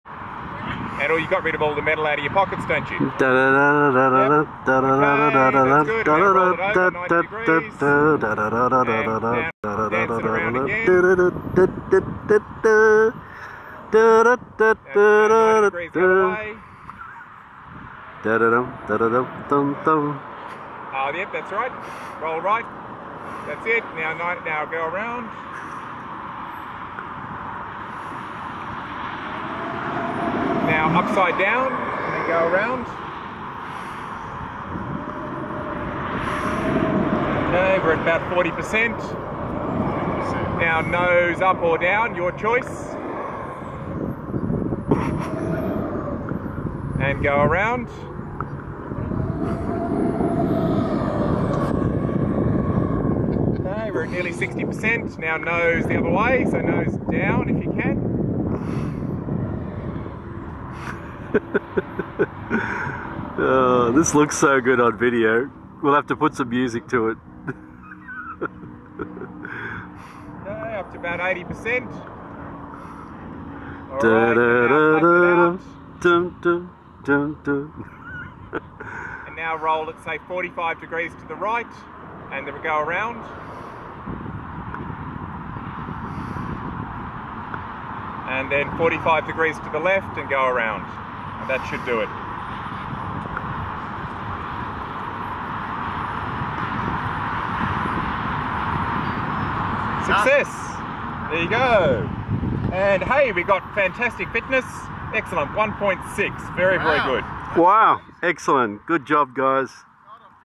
QuadPlane Compass Dance
Calibrating the compass on a large ArduPilot QuadPlane